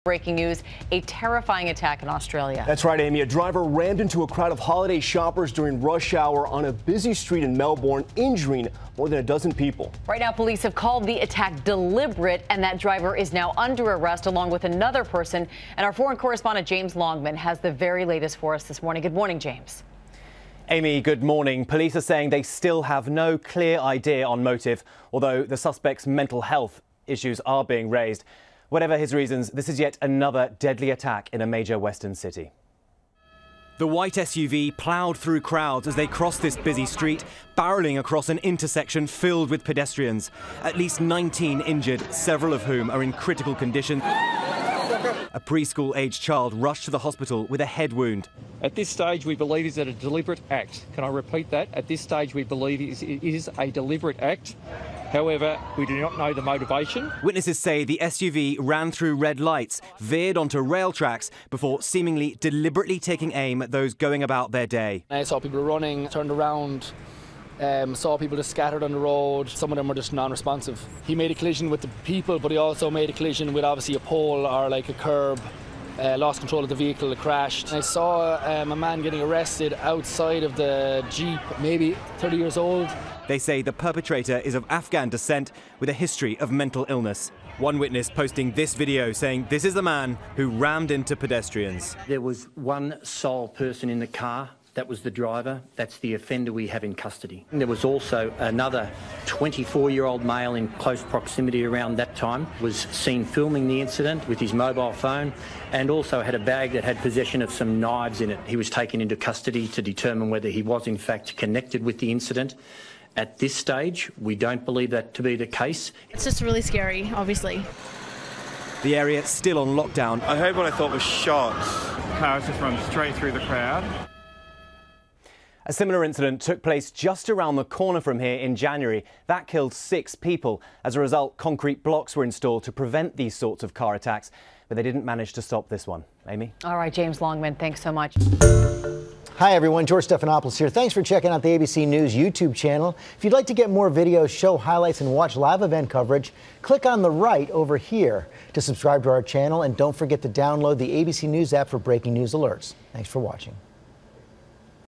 'Deliberate' car ramming injures 19 in Australia
A vehicle plowed into shoppers in Melbourne's central business district in what police called a "deliberate" and "cowardly" act.